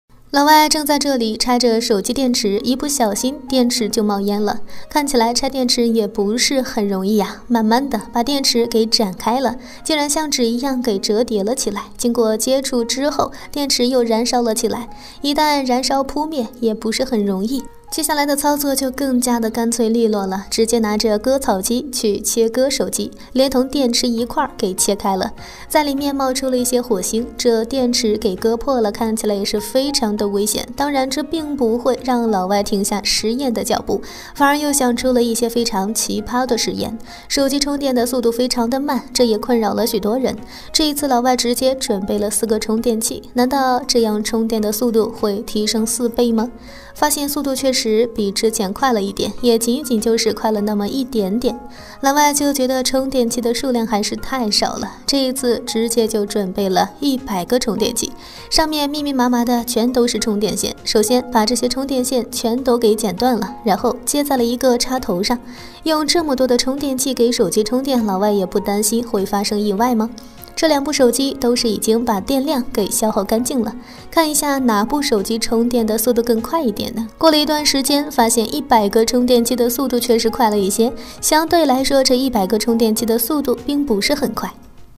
100个充电器同时给手机充电，充电速度能变快吗？结局太意外了！
＃ 盼盼黑科技